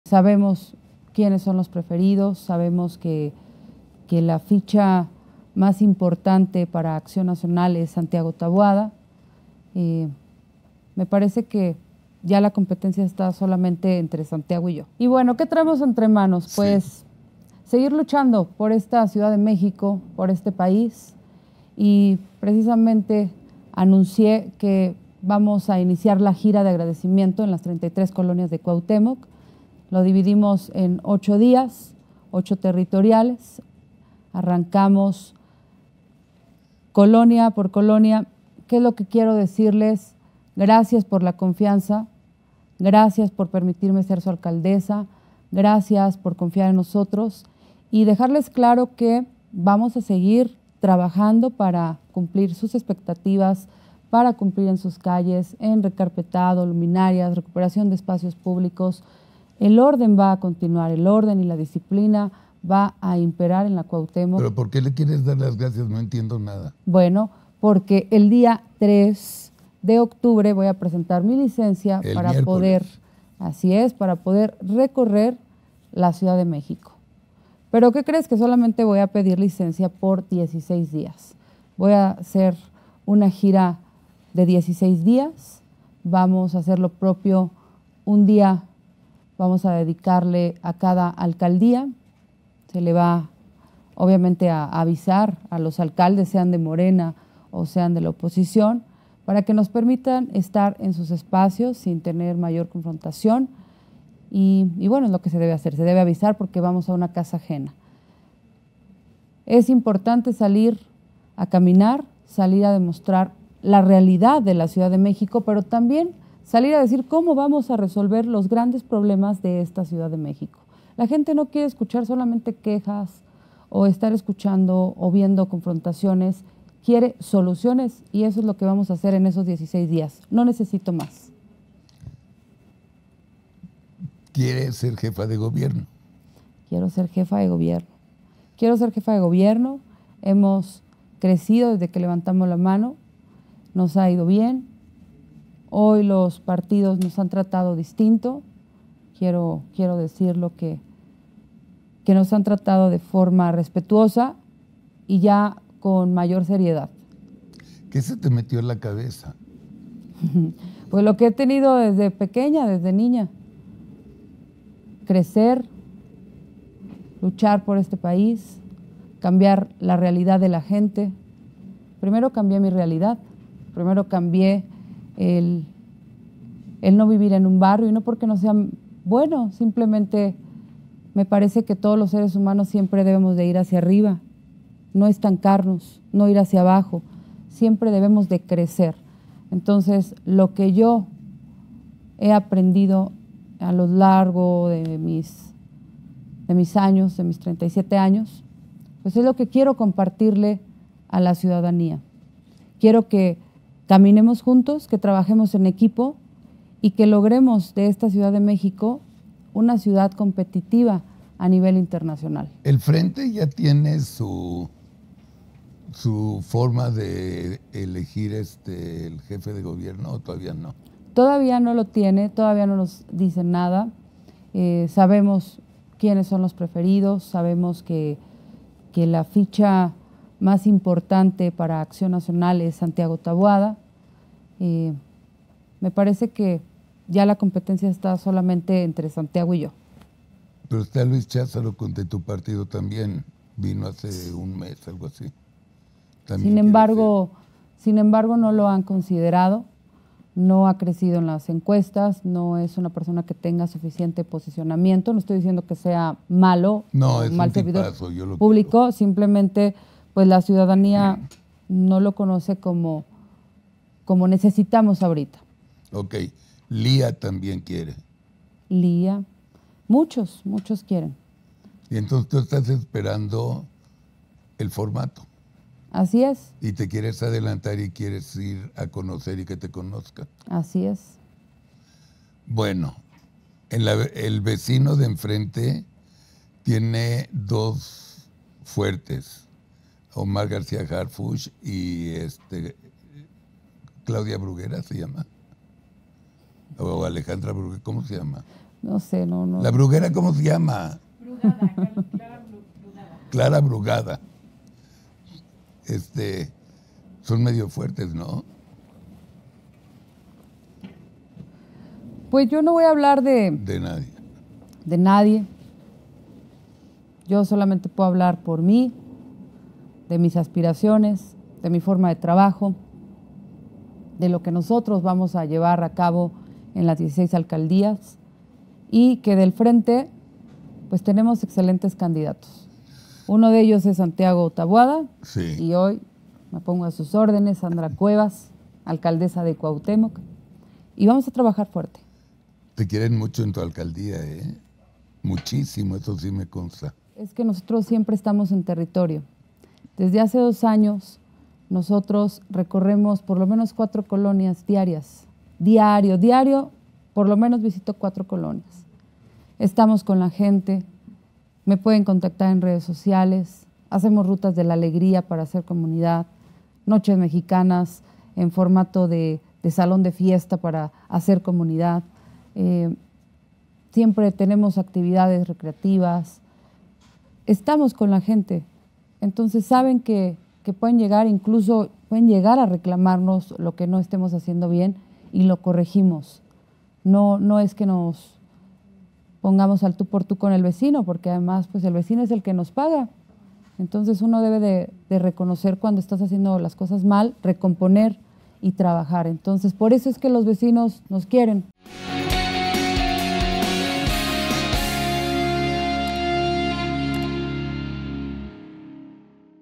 " HOY PEDIRÉ LICENCIA": SANDRA CUEVAS está CONFIADA en SER LA CANDIDATA del FRENTE para la CDMX
⭐️ APÓYANOS CON TU DONATIVO PARA QUE ATYPICAL CONTINÚE:
️Banca Mifel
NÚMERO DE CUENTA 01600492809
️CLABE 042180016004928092

Sandra Cuevas iniciará una gira de despedida en la Alcaldía Cuauhtémoc con miras a la elección del 2024

► ¿QUIERES ANUNCIARTE?